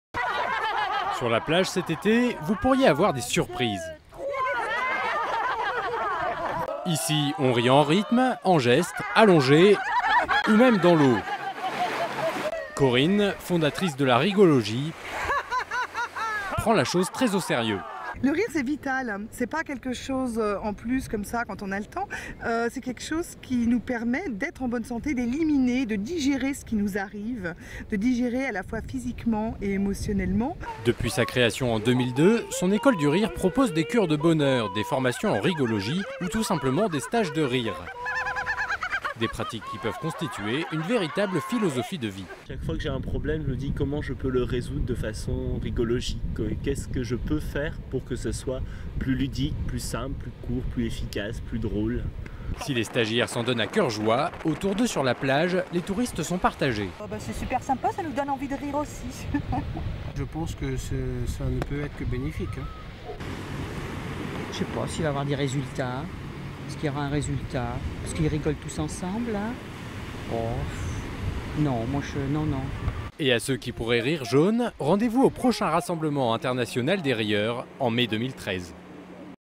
Rigologie à la plage
Petite séance de rigologie sur la plage de Frontignan avec Corinne Cosseron et Jullien Peschot